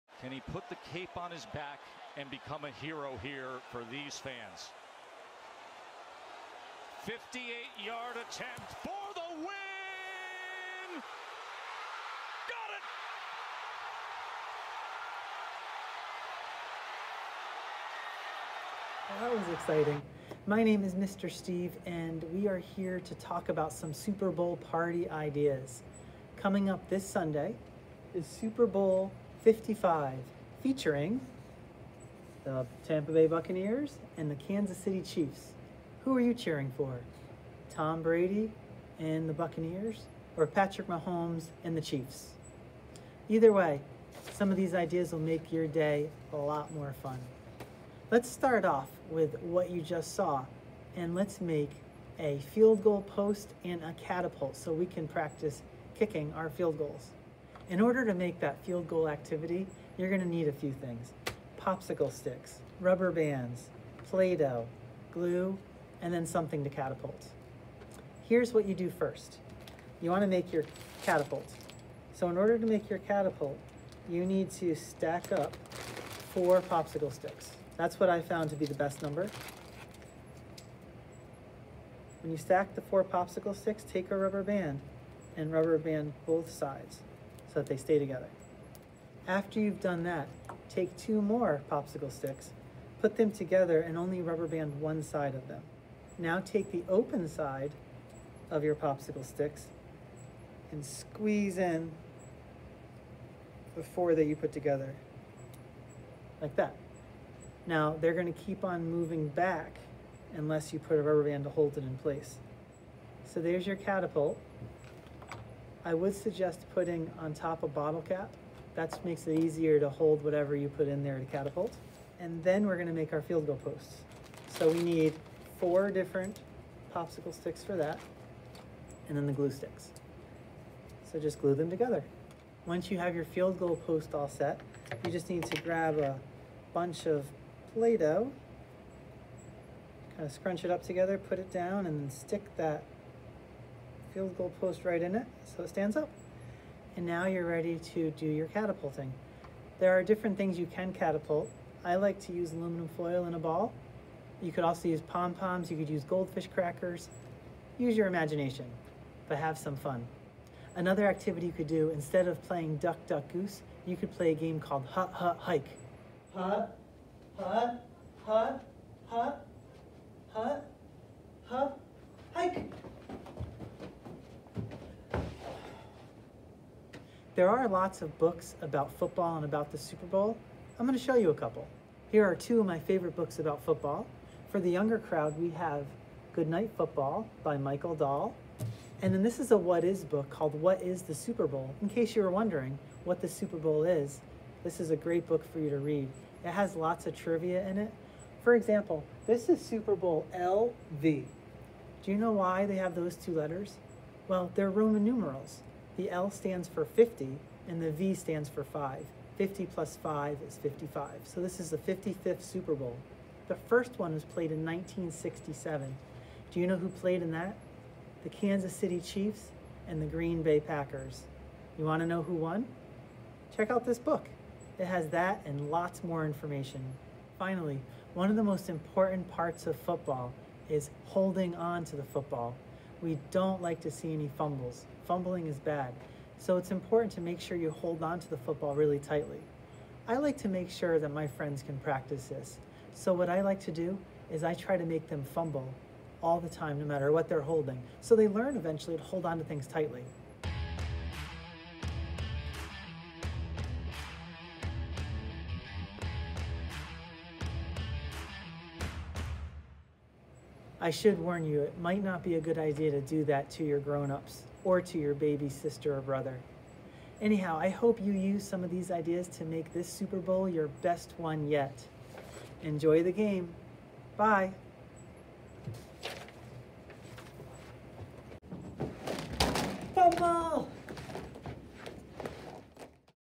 Super Bowl Party Ideas for Kids
Try these fun activities to make a happy Super Bowl - no matter which team wins!